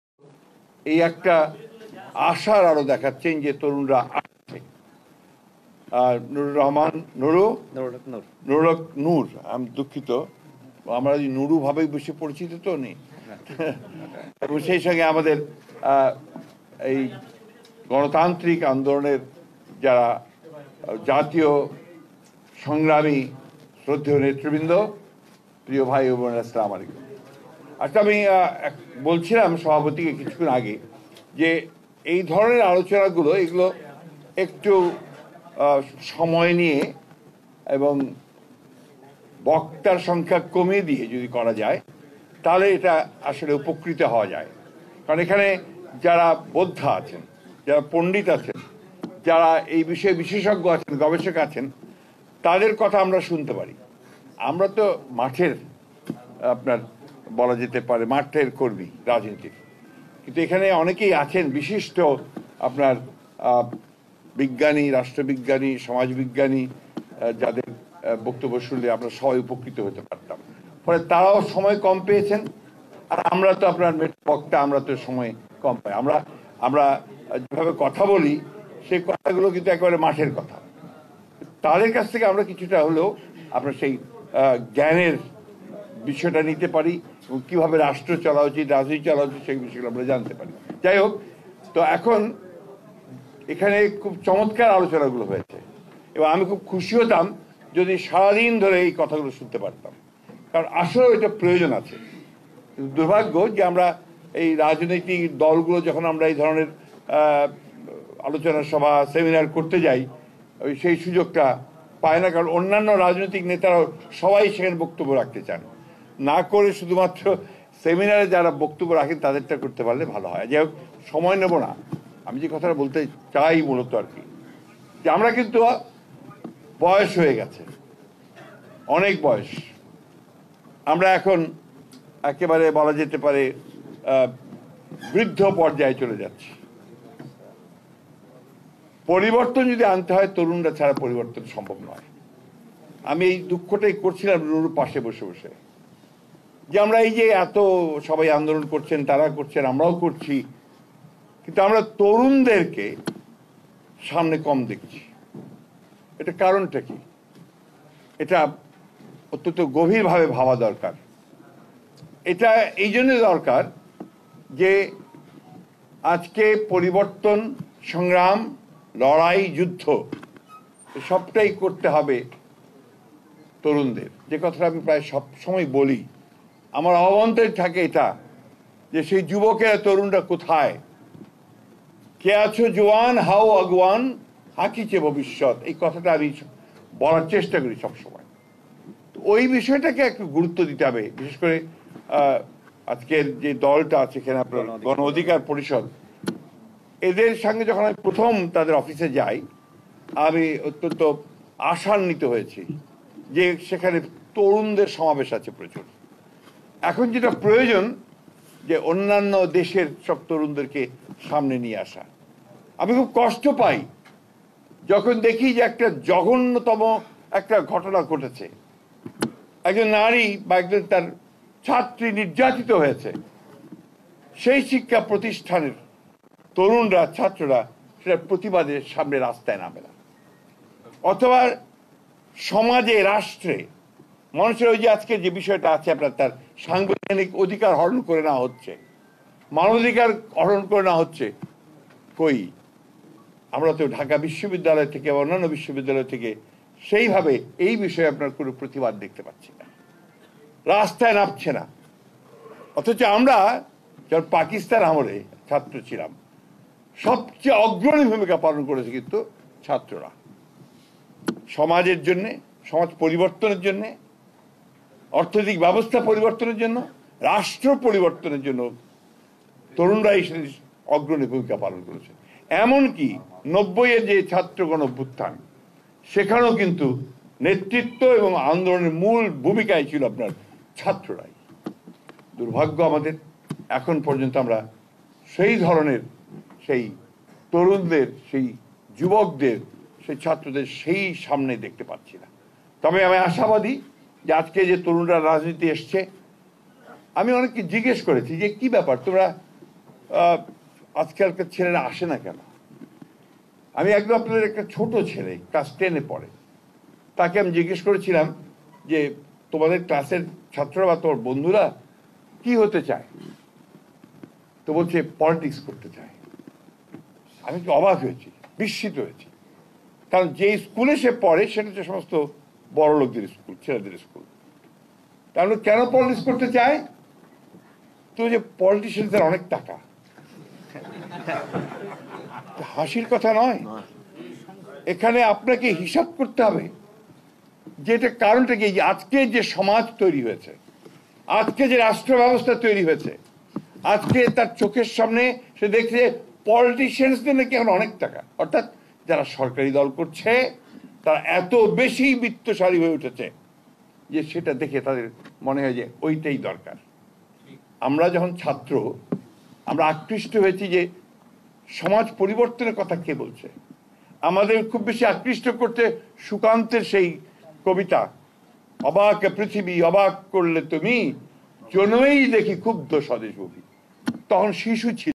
আওয়ামী লীগ দেশের মালিক মনে করে: সংগ্রামে তরুণদের ভূমিকা বেশি থাকে
নাগরিকদের সাংবিধানিক,মৌলিক ও মানবাধিকার সুরক্ষা শীর্ষক আলোচনা সভায় কথা বলছেন বিএনপির মহাসচিব মির্জা ফখরুল ইসলাম আলমগীর।